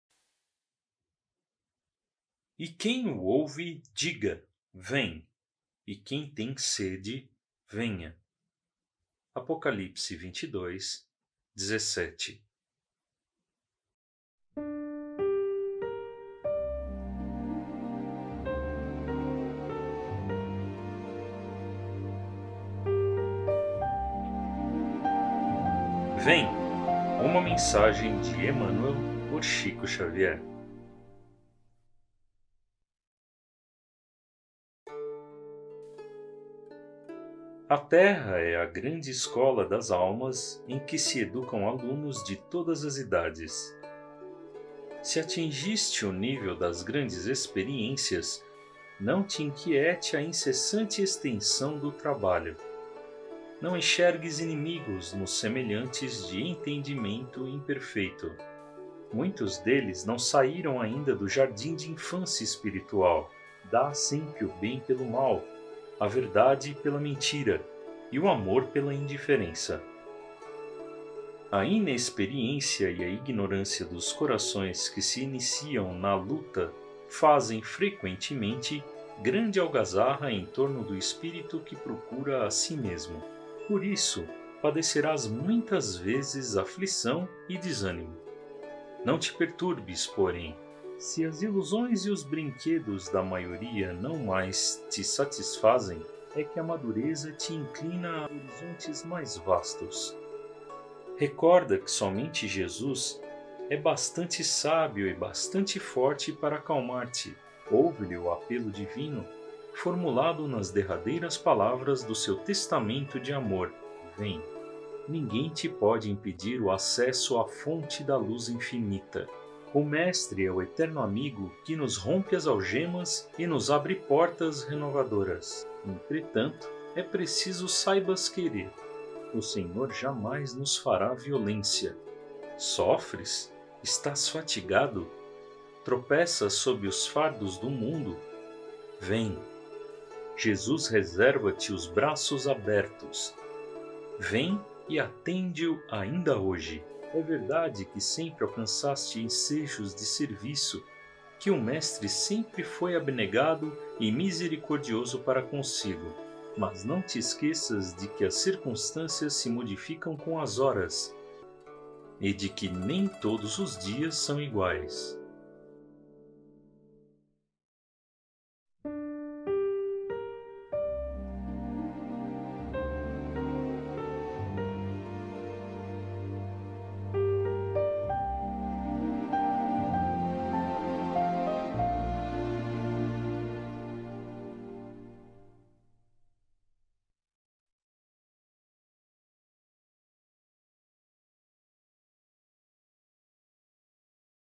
“VEM!”, de Emmanuel, por Chico Xavier - Mensagens nº 96
“ 'E quem o ouve, diga:
Vem. E quem tem sede, venha'
(Apocalipse, 22:17.)

A Terra é a grande escola das almas em que se educam alunos de todas as idades.

Se atingiste o nível das grandes experiências, não te inquiete a incessante extensão do trabalho.

Não enxergues inimigos nos semelhantes de entendimento imperfeito. Muitos deles não saíram ainda do jardim de infância espiritual.

Dá sempre o bem pelo mal, a verdade pela mentira e o amor pela indiferença…

A inexperiência e a ignorância dos corações que se iniciam na luta fazem, frequentemente, grande algazarra em torno do espírito que procura a si mesmo.

Por isso, padecerás muitas vezes aflição e desânimo.

Não te perturbes, porém.

Se as ilusões e os brinquedos da maioria não mais te satisfazem, é que a madureza te inclina a horizontes mais vastos.

Recorda que somente Jesus é bastante sábio e bastante forte para acalmar-te.

Ouve-lhe o apelo divino, formulado nas derradeiras palavras do seu Testamento de Amor: - "Vem!"

Ninguém te pode impedir o acesso à fonte da luz infinita.

O Mestre é o Eterno Amigo que nos rompe as algemas e nos abre portas renovadoras... Entretanto, é preciso saibas querer.

O Senhor jamais nos fará violência.

Sofres? Estás fatigado? Tropeças sob os fardos do mundo? Vem!

Jesus reserva-te os braços abertos.

Vem e atende-o ainda hoje. É verdade que sempre alcançaste ensejos de serviço, que o Mestre sempre foi abnegado e misericordioso para contigo, mas não te esqueças que as circunstâncias se modificam com as horas e de que nem todos os dias são iguais."

Emmanuel
Da obra: "Doutrina e Aplicação" (1989)
Psicografado por Francisco Cândido Xavier

Locução: Prado.

▶ Visite nossa página no FB: @umplanomaior

▶ 04/05/2020 + 28/05/2021